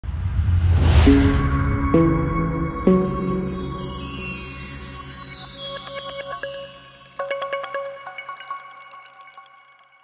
50º aniversário TAMA
A TAMA está animada em comemorar seu 50º aniversário! Temos muitas novidades legais reservadas para 2024, então fique de olho em nossos canais de mídia social: @tamadrums_brasil.